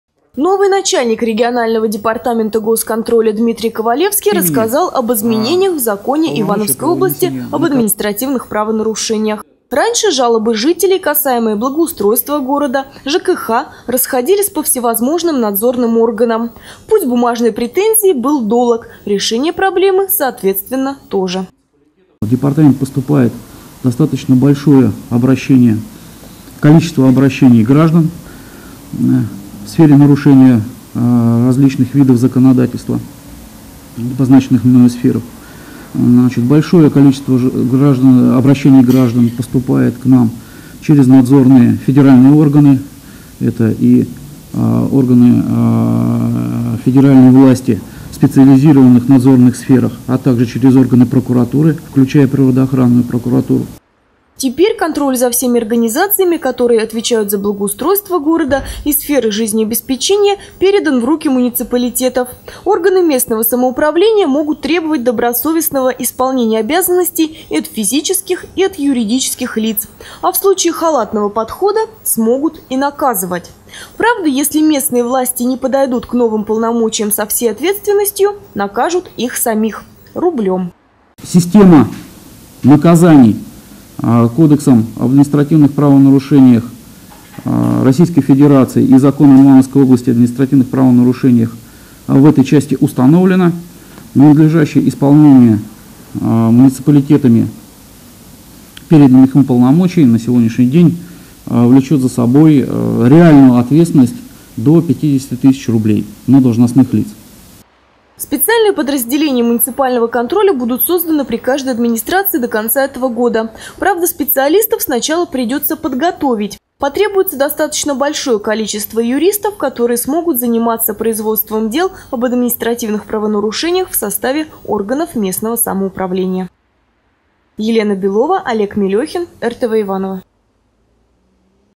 Административные правонарушения под контролем
Новый начальник регионального департамента госконтроля Дмитрий Ковалевский рассказал об изменениях в законе Ивановской области об административных правонарушениях